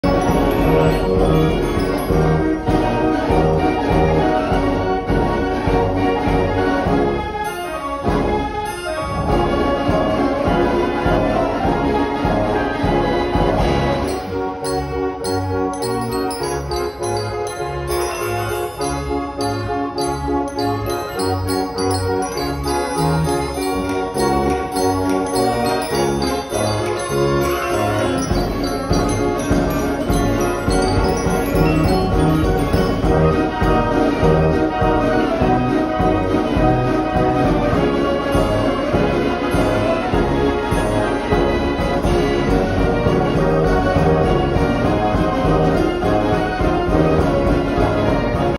This Self Playing Orchestra from Coney island
recovered from a mass fire on Coney Island